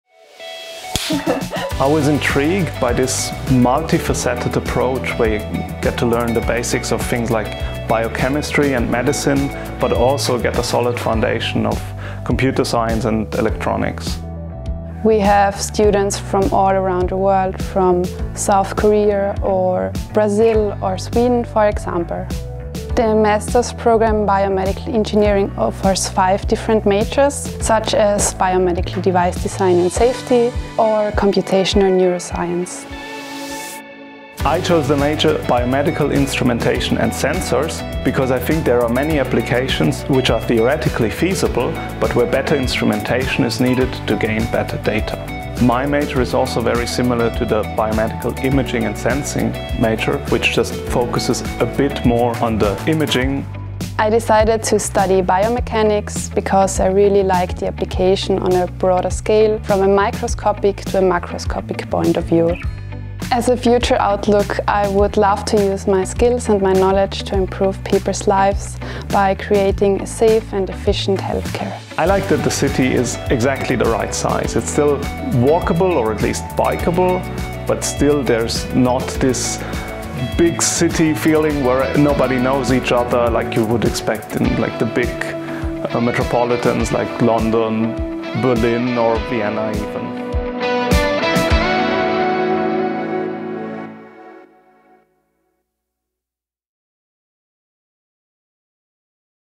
Biomedical Engineering @ TU Graz
TU Graz offers 5 majors within the master's degree programme Biomedical Engineering:

Biomechanics - Modeling and Simulation
Biomedical Instrumentation and Sensors
Biomedical Imaging and Sensing
Computational Neuroscience
Biomedical Device Design, Safety and Regulation